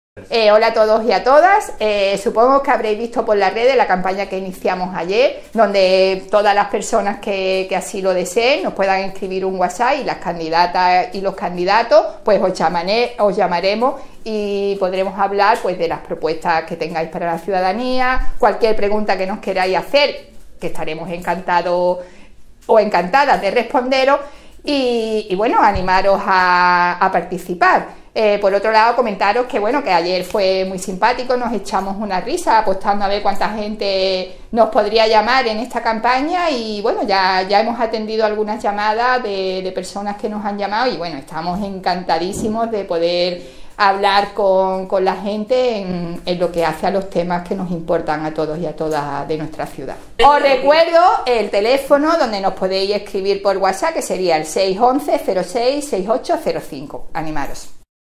Escríbenos al whatsapp
Ayer iniciamos una campaña por redes con lo idea de que cualquier persona pueda escribirnos por whatsApp pasar su teléfono de contacto y estaremos encantadas o encantados de contestar a cualquier pregunta que quieran hacernos. Y como no, recibir cualquier propuesta que hagan a la mejora de Málaga. No te cortes un pelo y escríbenos!!